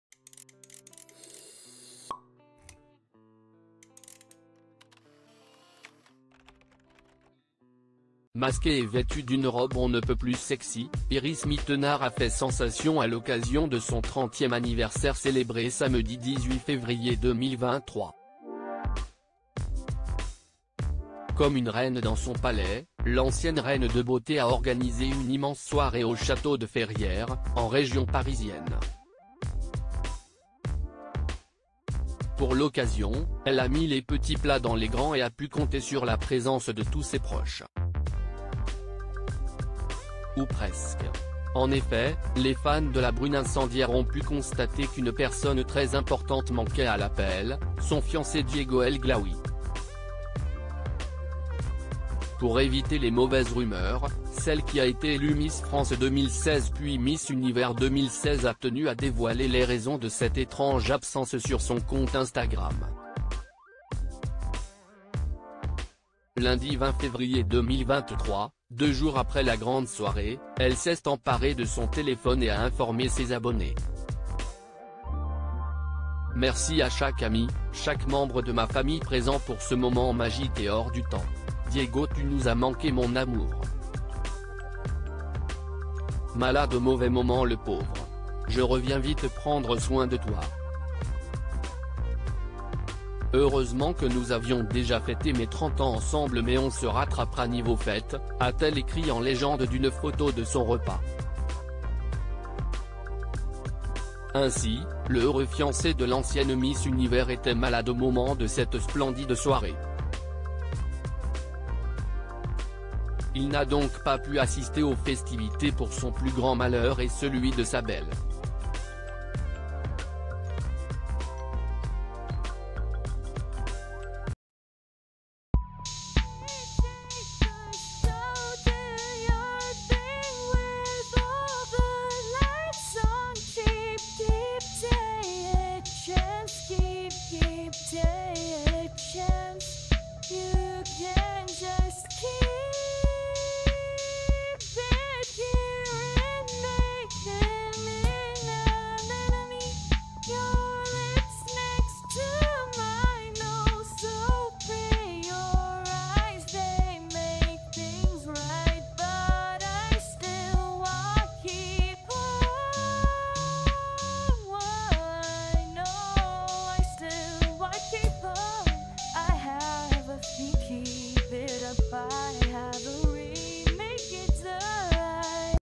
"Le pauvre !" : Iris Mittenaere révèle pourquoi son fiancé Diego El Glaoui était absent à son annive
"Le pauvre !" : Iris Mittenaere révèle pourquoi son fiancé Diego El Glaoui était absent à son anniversaire royal